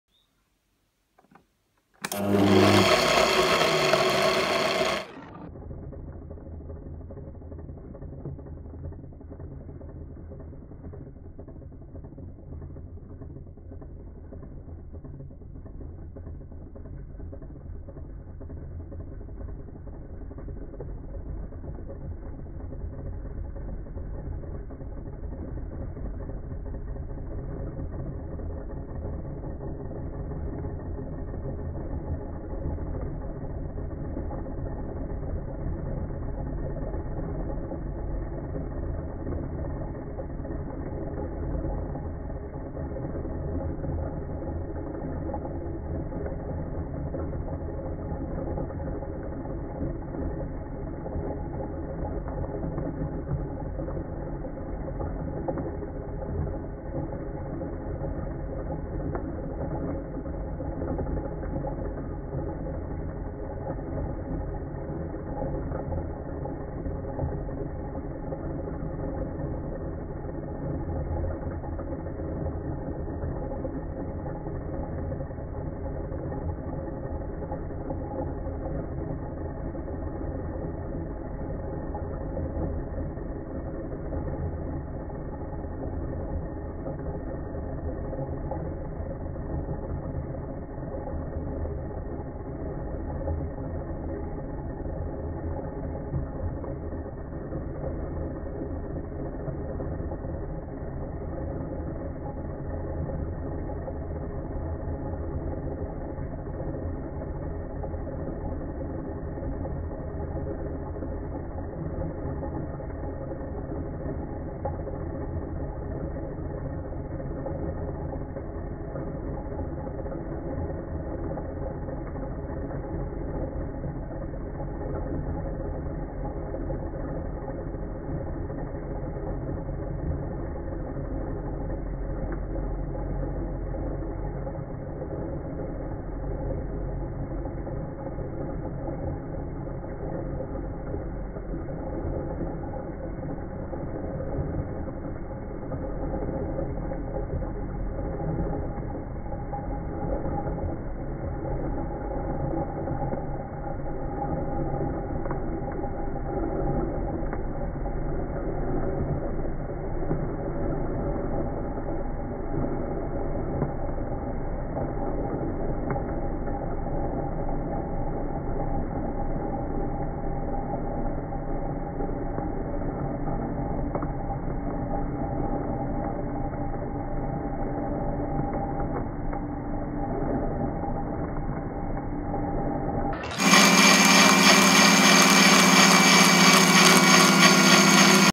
Vanilleextrakt selber machen
Vanilleextrakt selber machen mit dem Magnetic Drive Blender von Kitchenaid.